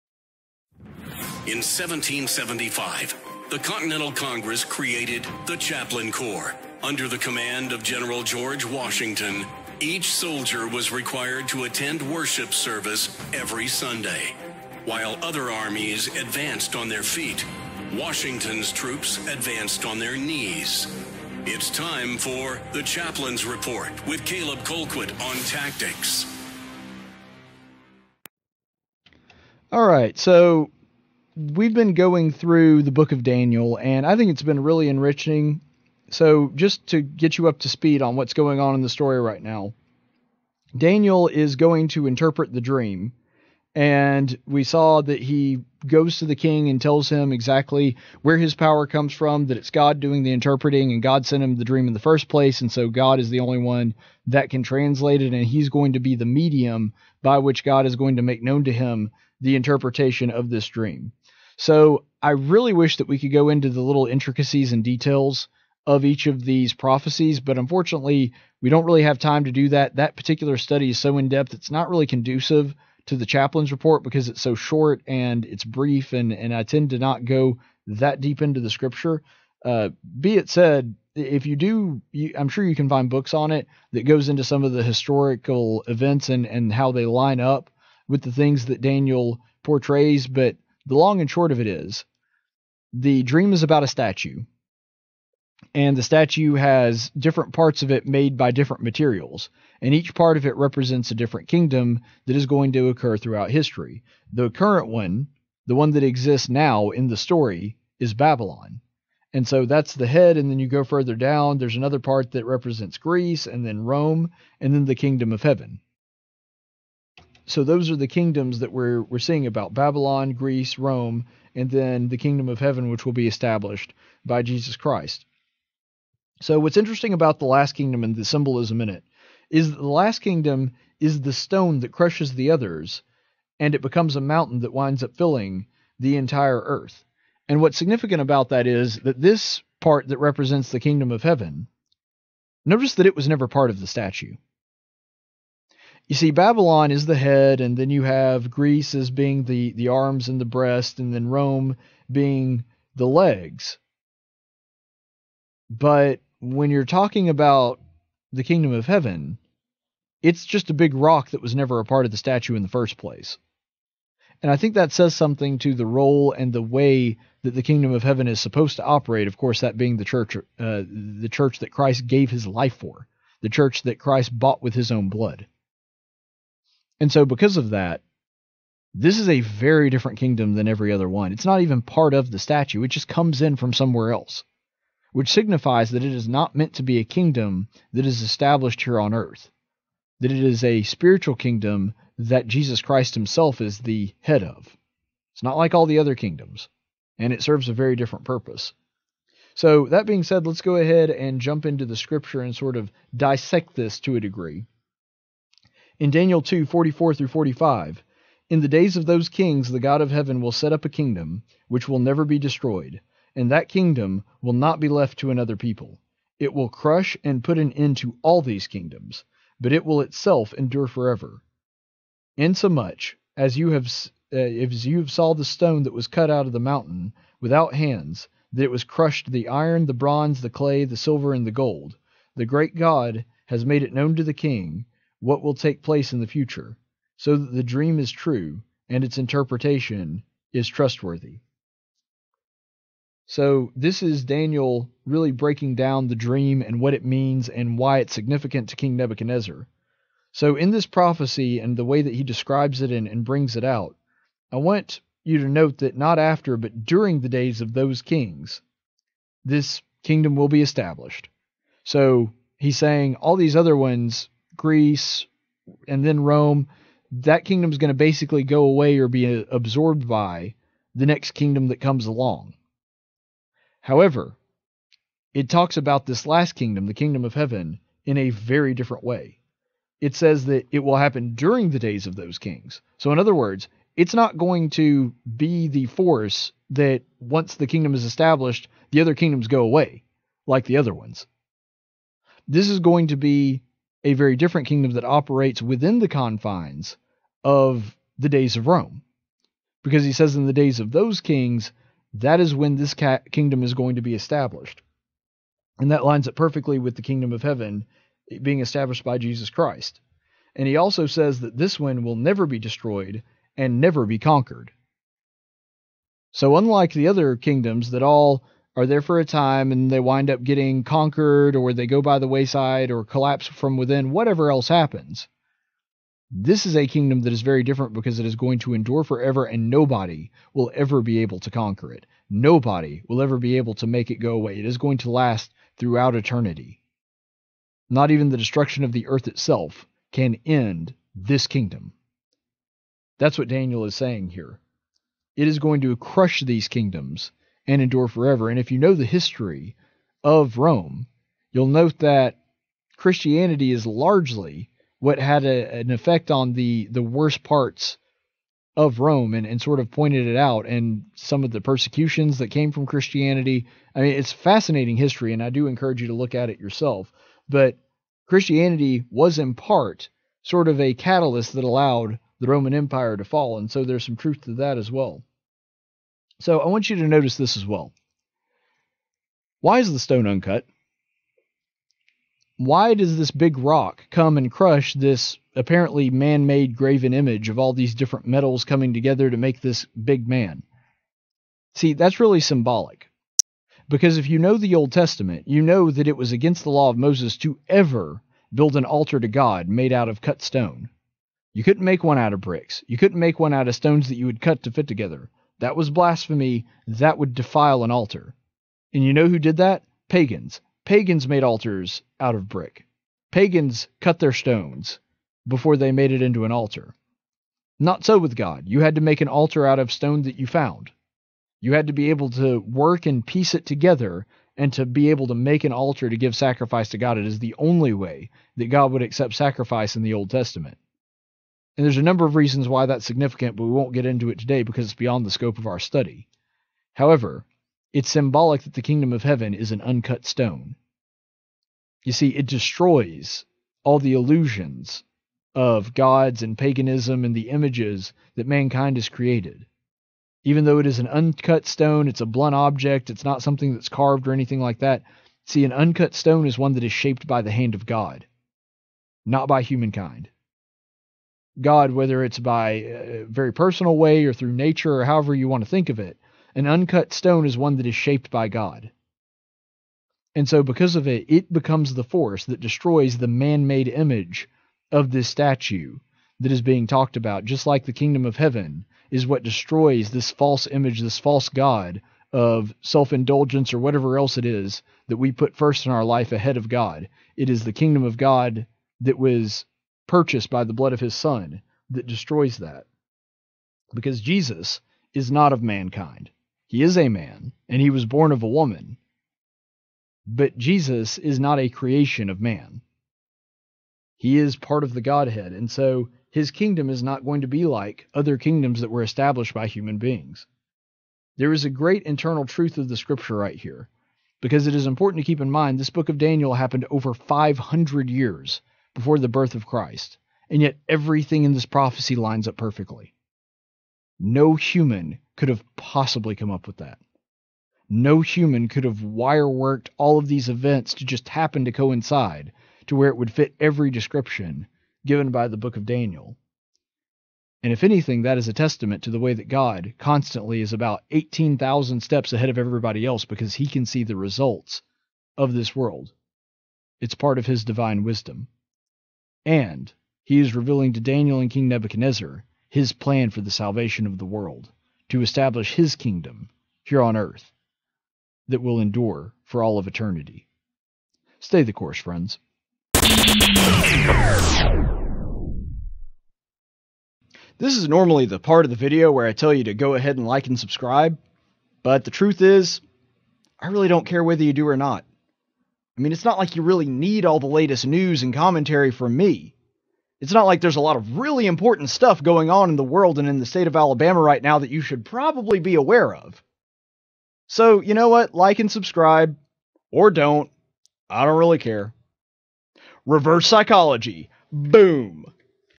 Chaplain's Report- The Coming Kingdom
Daniel interprets the King's dream and reveals it is a prophesy of four kingdoms in the future.  We find the final kingdom described in Daniel 2:44-45 is very different from the previous ones.